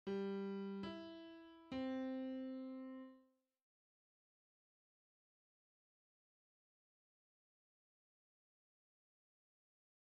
NBC Logo made in FL Studio
I install the full version of FL Studio 20

It's looks pretty good btw.